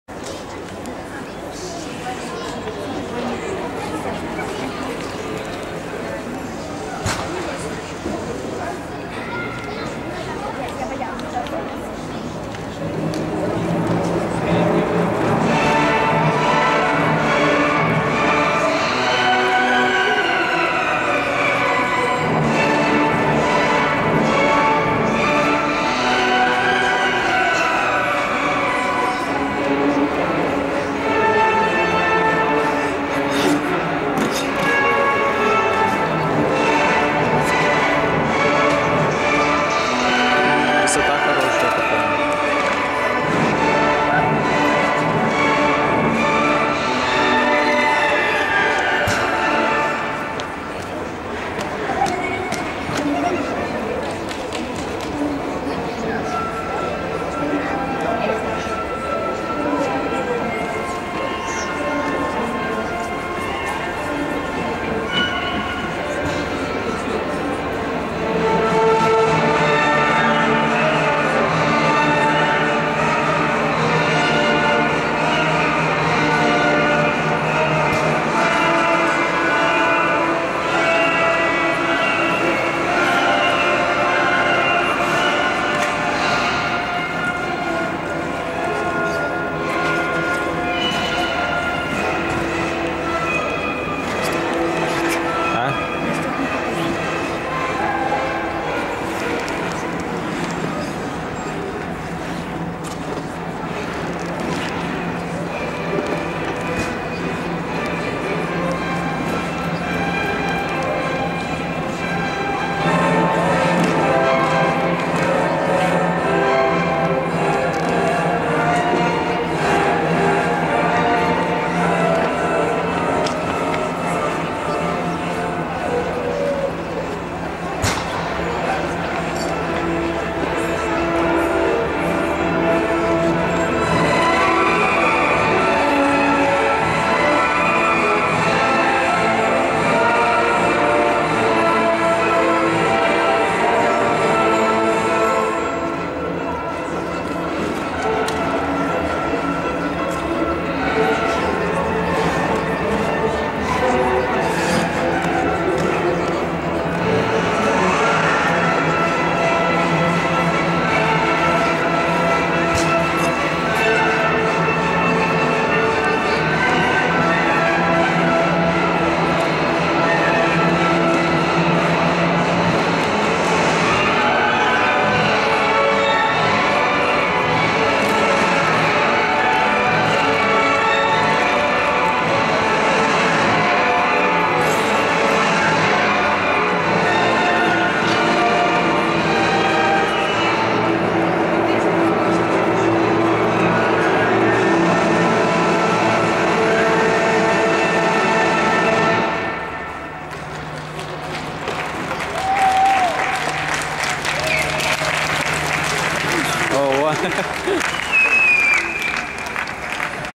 Ксения Евдокименко - Харьков ПП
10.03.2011 г. Мариуполь.  4-й Этап. Кубок Украины юношеского возраста по фигурному катанию на коньках.  Первенство г. Мариуполя